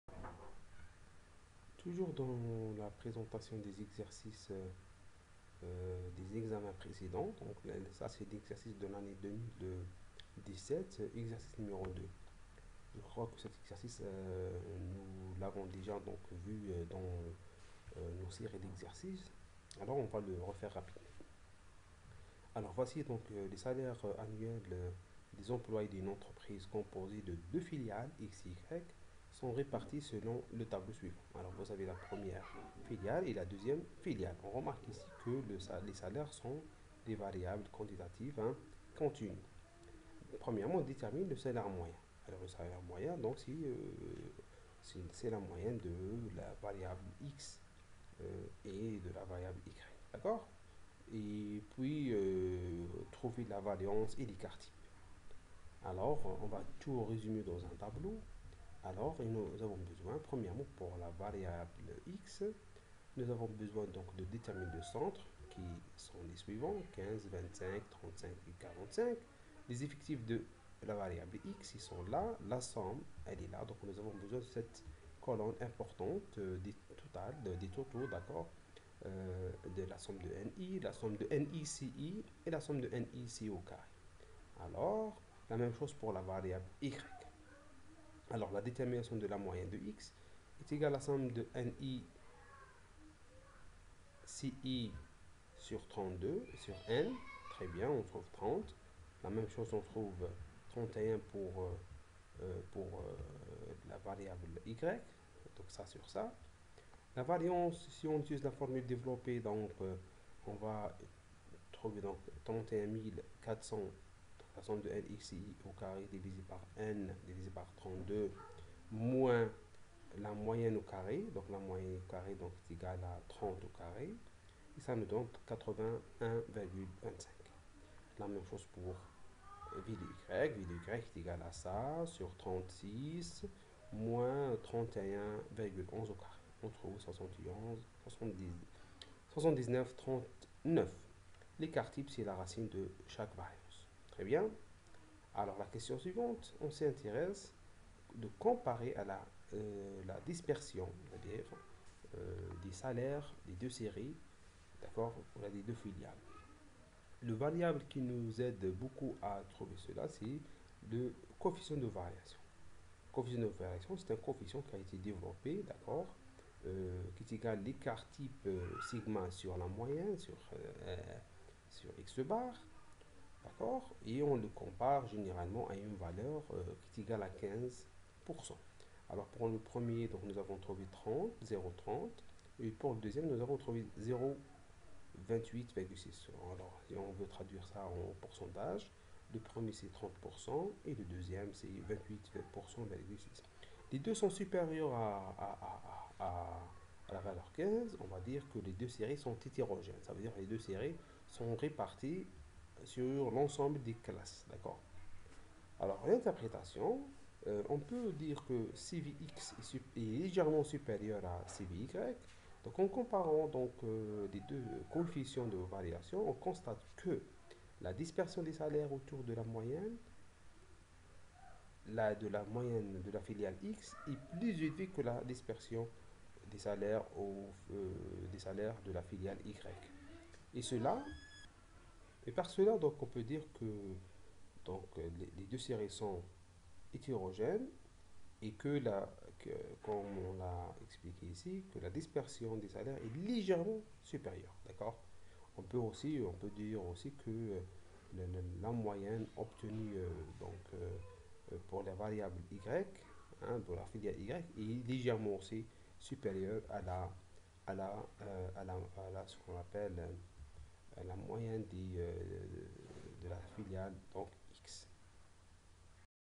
N°93: Corrigé de l'exercice n°2 d'examen Stat ENP "2017" Moyenne/Variance/Coefficient de variation
Moyenne/Variance/Coefficient de variation
الإحصاء الوصفي